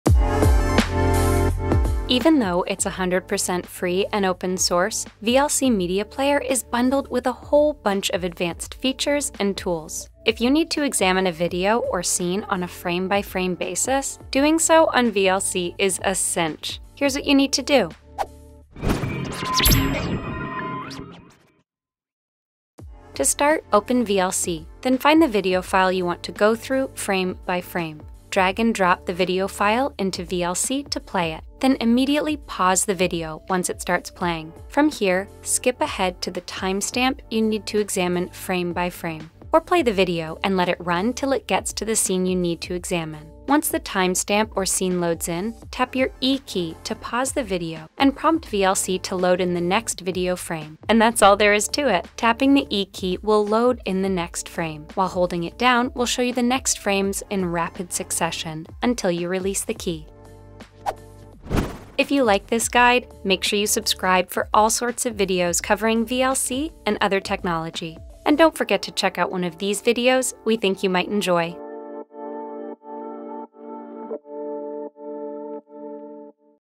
How to Go Through Video Frame by Frame with VLC Media Player
Despite being 100% free and open source, VLC media player is bundled with a whole bunch of advanced features and tools. If you need to examine a video or scene on a frame by frame basis, doing so on VLC is a cinch! Here’s what you need to do.